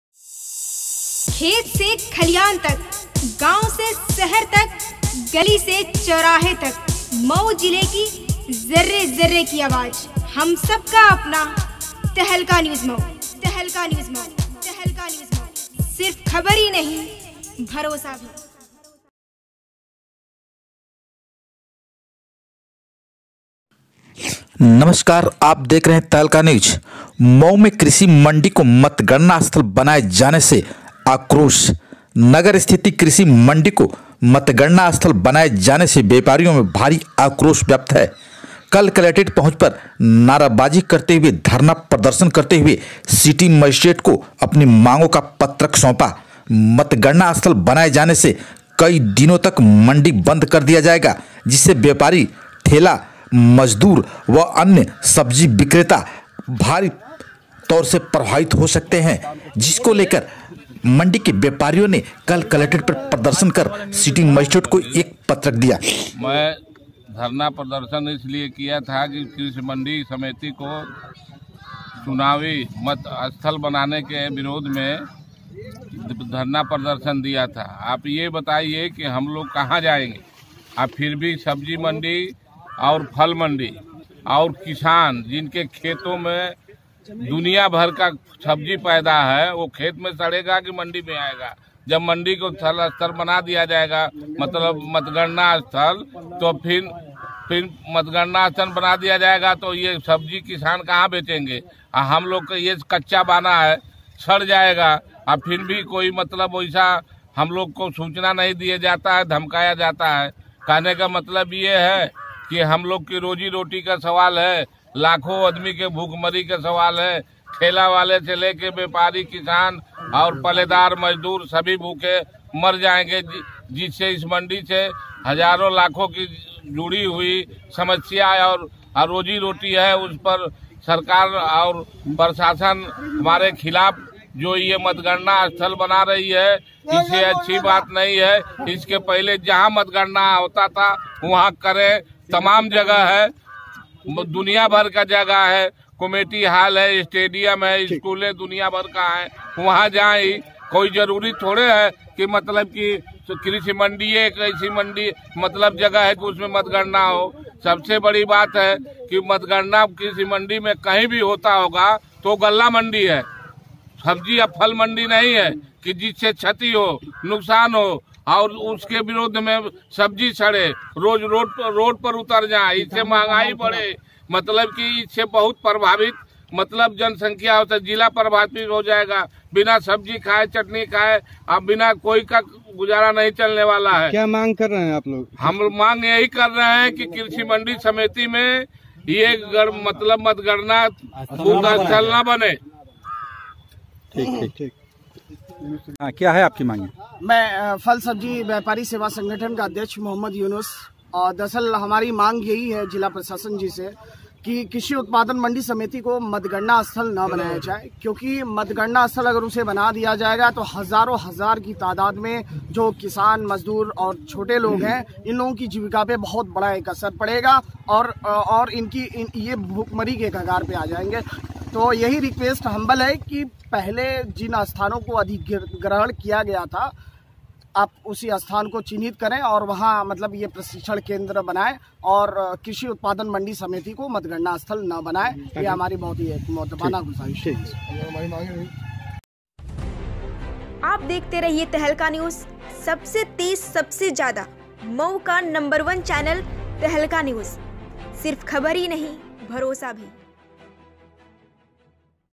मऊ में कृषि मंडी को मतगणना स्थल बनायें जाने से आक्रोश#
मऊ/नगर स्थिति कृषि मंडी को मतगणना स्थल बनायें जाने से व्यापारियों में भारी आक्रोश व्याप्त हैं।कलेक्ट्रेट पहुच कर नारेबाजी करते हुए धरना प्रदर्शन करते हुए सिटी मजिस्ट्रेट को पत्रक सौपा।
 मतगणना स्थल बनाएं जाने से कई दिनों तक मंडी को बन्द कर दिया जायेगा जिससे व्यापारी,ठेला,मजदूरी करने वालों से लेकर सब्जी,फल आदि बिक्री पूरी तरह से प्रभावित होगी।